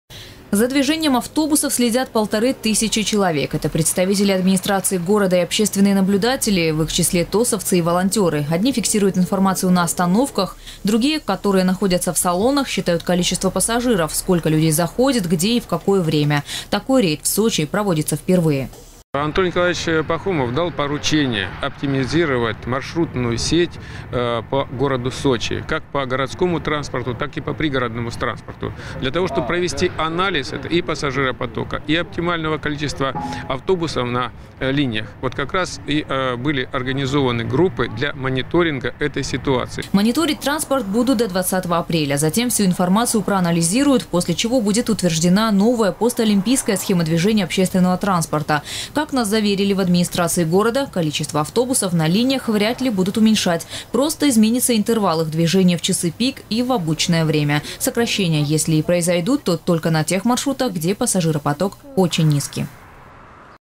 В Сочи началась тотальная проверка общественного транспорта. Новости 24 Сочи
Поверяют все: начиная от соблюдением графика, заканчивая количеством пассажиров.
Сочи. Новости.
Телеканал Эфкате.